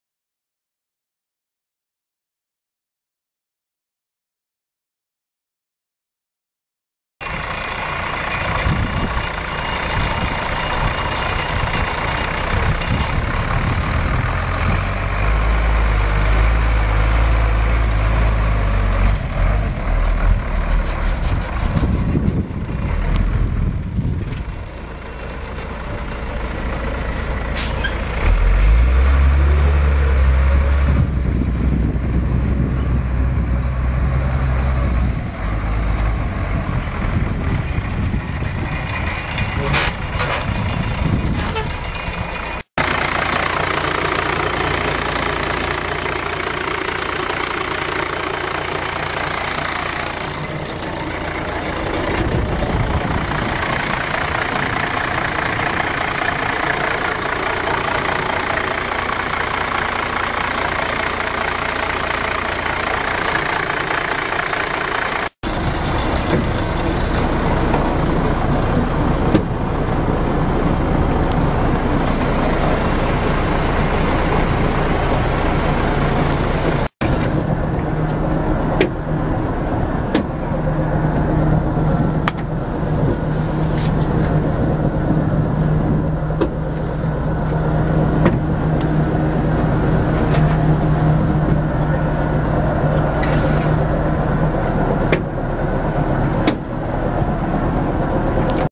1997 Ford Louisville feed truck with Roto-mix 600-16 mixer | no-reserve auction August 31, 2011
Absolute Internet auction by Purple Wave on August 31, 2011

1997 Ford Louisville feed truck with Roto-mix 600-16 mixer
114,826 miles on odometer
29,068 hours on meter
Cummins ISC 8.3L six cylinder diesel engine
Serial 4S440499
Five speed automatic transmission
AC and heat
AM/FM
Air ride seat
Chelsea PTO
Air brakes
Tyrel TCX-1000 digital scale
Star DP8340
Roto-Mix 600-16 mixer
Hydraulic spout and trap door
Stainless steel liner
Five bar rotor mixer
Dual auger
600 cu. ft. mix ration capacity
17,000 lb load weight capacity
Serial 49027
11R22.5 tires
208" wheelbase
1FDXS80E1VVA35610
Seguin, Texas